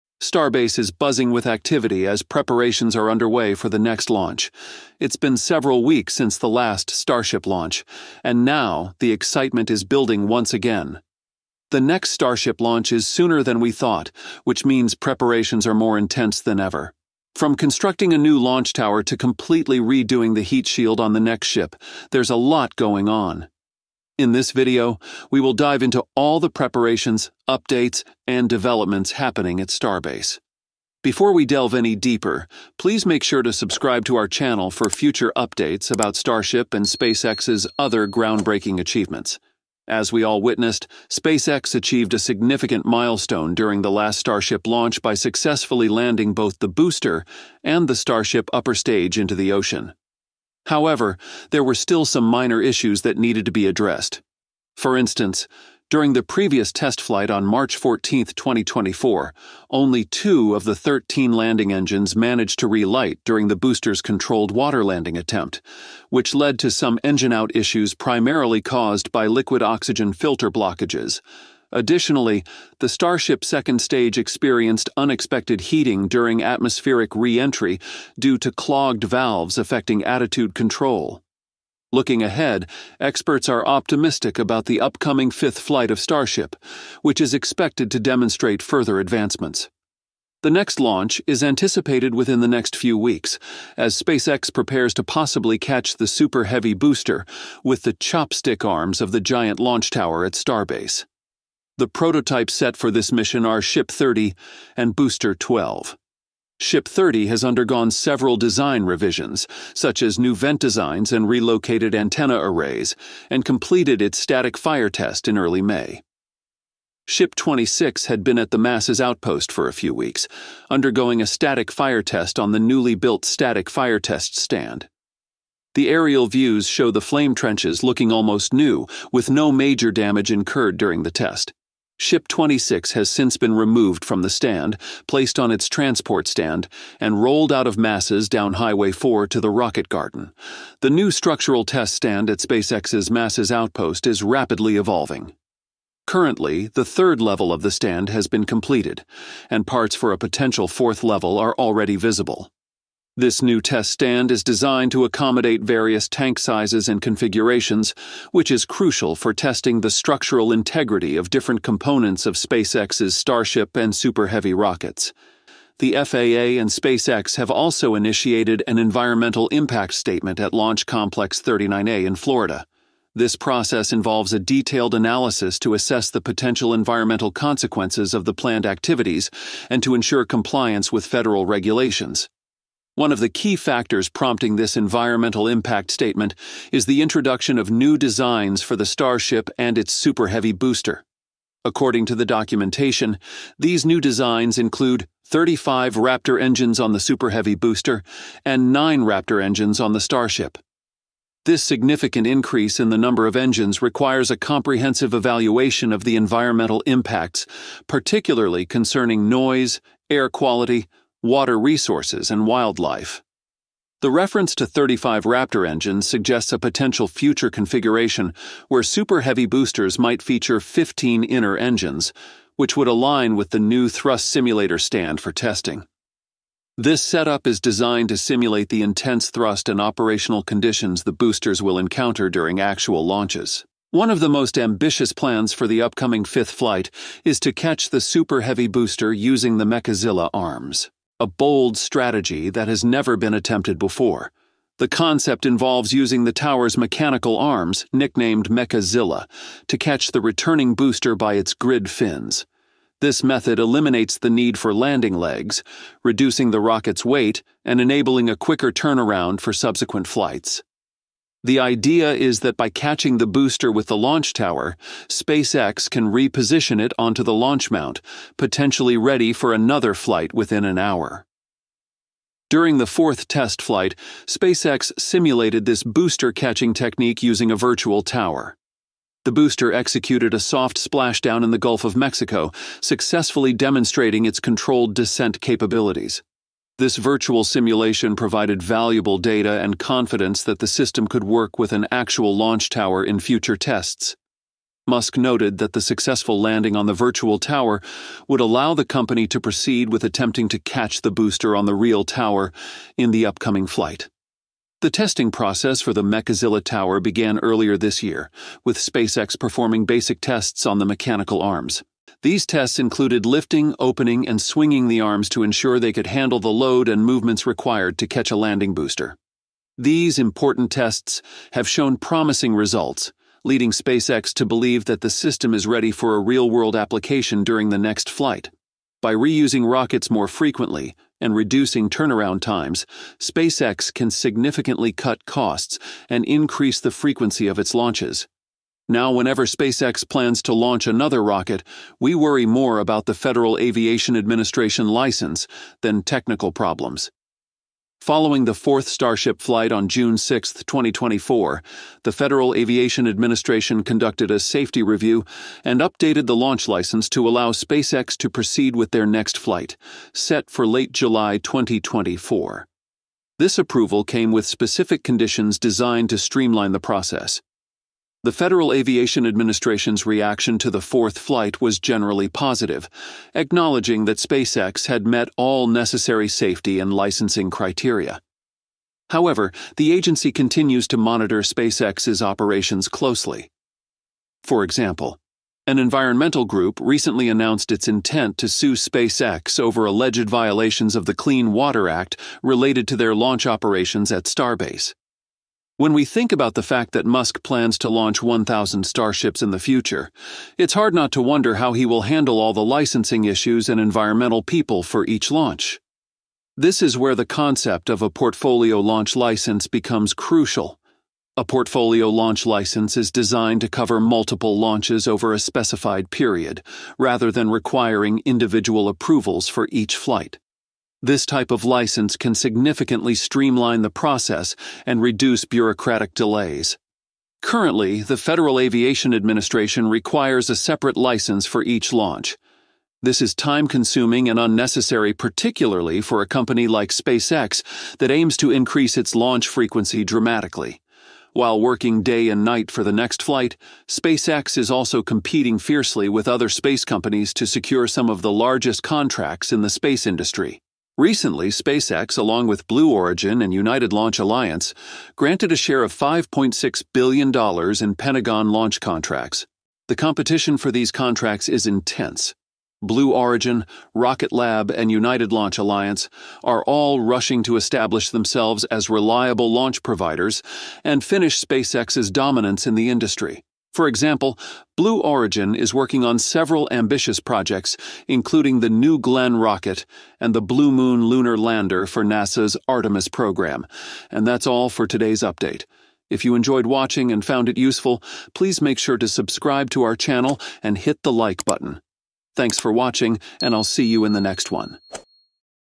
Intense Preparations For The 5th Starship Launch....Important Upgrades!
Starbase is buzzing with activity as preparations are underway for the next launch. It’s been several weeks since the last Starship launch, and now, the excitement is building once again. The next Starship launch is sooner than we thought, which means preparations are more intense than ever. From constructing a new launch tower to completely redoing the heat shield on the next ship, there’s a lot going on.
In this video, we will dive into all the preparations, updates, and developments happening at Starbase.
As we all witnessed, SpaceX achieved a significant milestone during the last Starship launch by successfully landing both the booster and the Starship upper stage into the ocean. However, there were still some minor issues that needed to be addressed. For instance, during the previous test flight on March 14, 2024, only two of the thirteen landing engines managed to relight during the booster’s controlled water landing attempt, which led to some engine-out issues primarily caused by liquid oxygen filter blockages. Additionally, the Starship second stage experienced unexpected heating during atmospheric reentry due to clogged valves affecting attitude control.
Looking ahead, experts are optimistic about the upcoming fifth flight of Starship, which is expected to demonstrate further advancements. The next launch is anticipated within the next few weeks, as SpaceX prepares to possibly catch the Super Heavy booster with the "chopstick" arms of the giant launch tower at Starbase.
The prototypes set for this mission are Ship 30 and Booster 12. Ship 30 has undergone several design revisions, such as new vent designs and relocated antenna arrays, and completed its static fire test in early May.
Ship 26 had been at the Masses Outpost for a few weeks, undergoing a static fire test on the newly built static fire test stand. The aerial views show the flame trenches looking almost new, with no major damage incurred during the test. If there was any damage, it was repaired quickly. Ship 26 has since been removed from the stand, placed on its transport stand, and rolled out of Masses down Highway 4 to the Rocket Garden.
The new structural test stand at SpaceX's Masses Outpost is rapidly evolving. Currently, the third level of the stand has been completed, and parts for a potential fourth level are already visible. This new test stand is designed to accommodate various tank sizes and configurations, which is crucial for testing the structural integrity of different components of SpaceX's Starship and Super Heavy rockets.
This stand will be used to conduct rigorous tests on tanks like the B1 14.1 test tank, which had a cap with a pusher structure to test the structural strength of the booster ahead of catches.